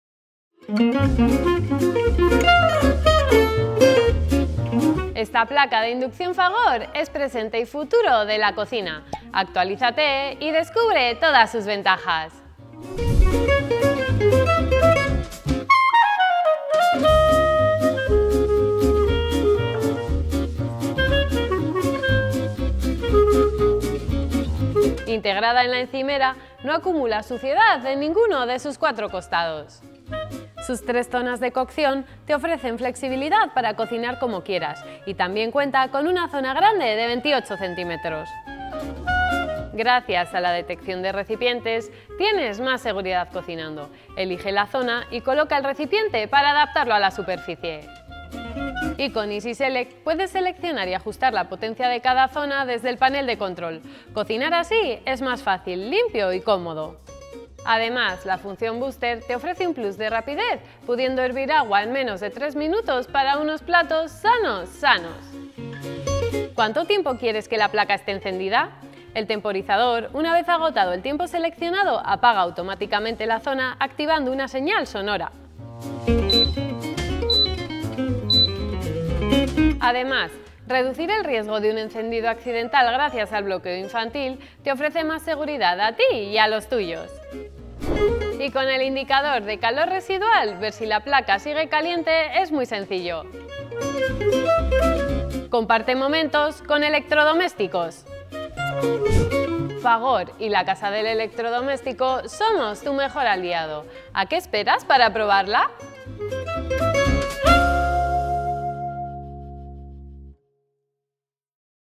🌡Fagor encimera de inducción | 3IF-305BC
🏡🏠 Desde _La Casa del Electrodoméstico_ te traemos esta práctica *🌡ENCIMERA DE INDUCCIÓN 🌡 negra FAGOR 3IF-305BC* de 60 cm. Cuenta con 3 zonas de cocción, 7200 w, entre ellas, una zona grande de 28 cm. 🥘
⬇️ Contenido: ⬇️

0:00 - Presentación
0:14 - Características técnicas
0:22 - Diseño y zonas
0:48 - EasySelect
0:57 - Booster
1:06 - Temporizador
1:19 - Bloqueo infantil
1:28 - Indicador de calor residual
1:34 - Despedida